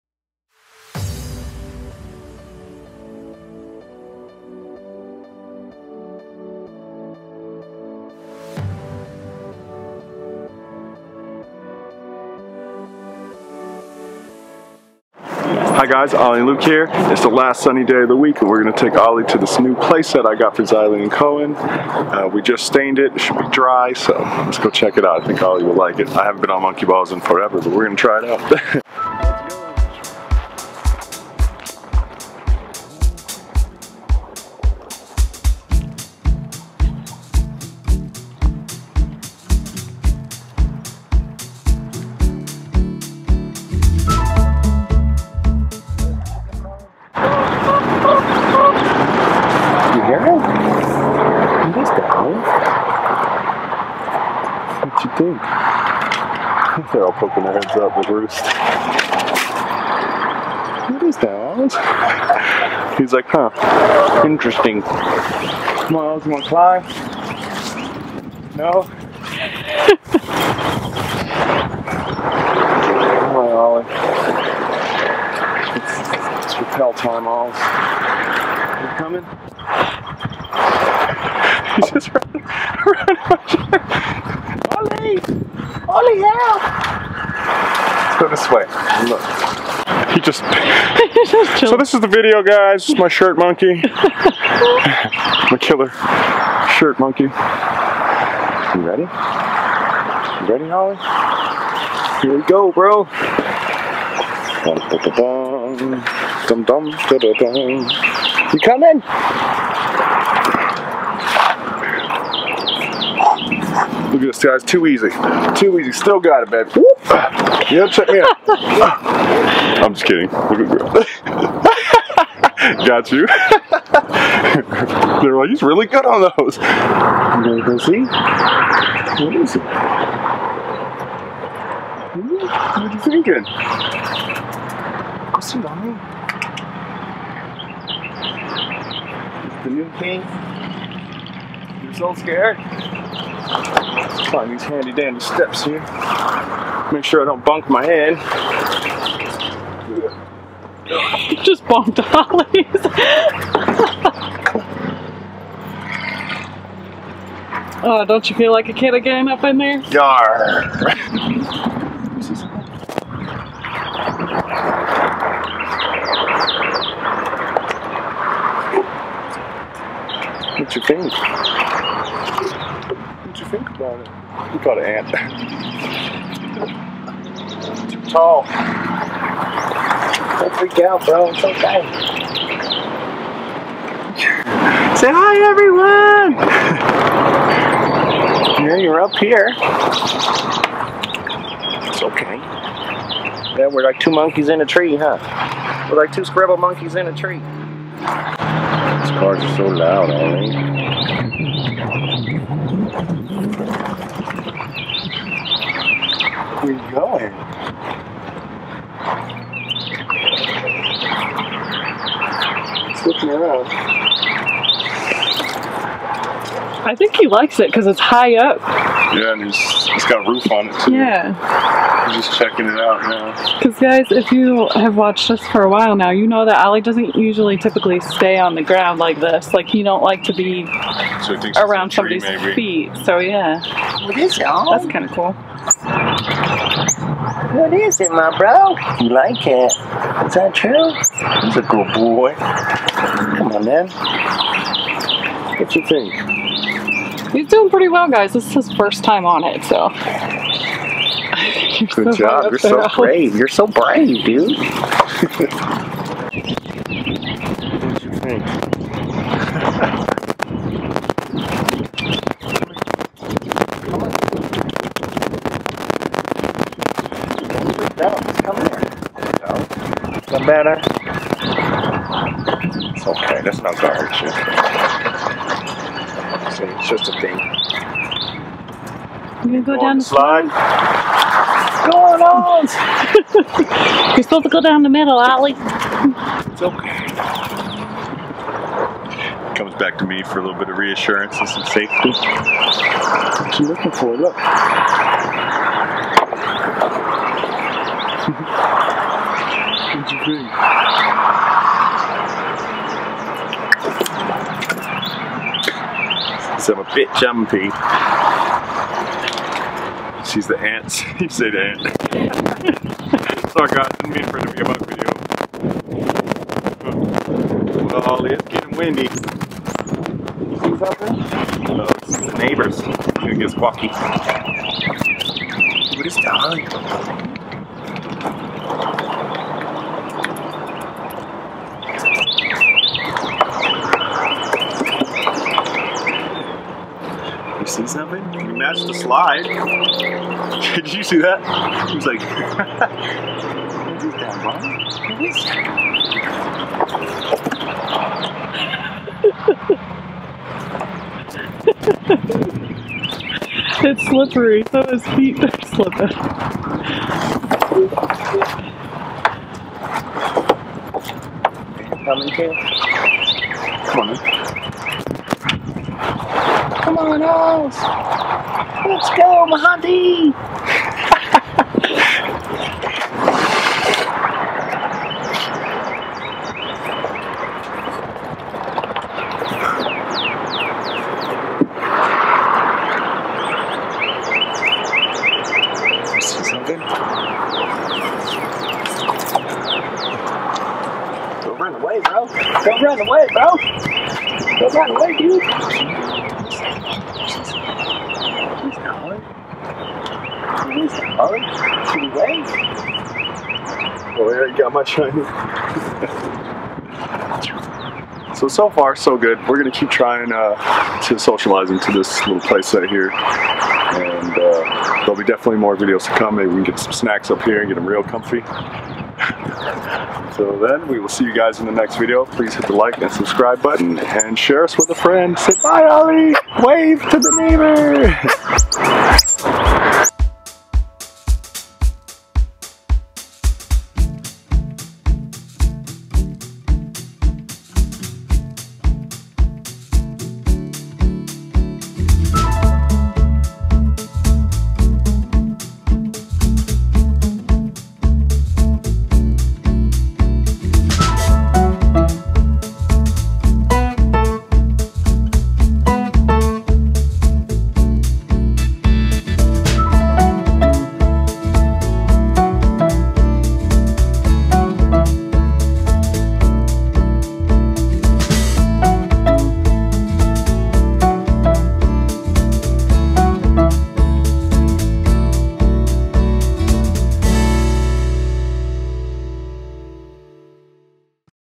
Baby Monkey oLLie Explores HUGE Play Set With Castle & Monkey Bars
SEND oLLie MAIL!!

If you read all of this, comment Castle MONKEY !!” below 🐒💕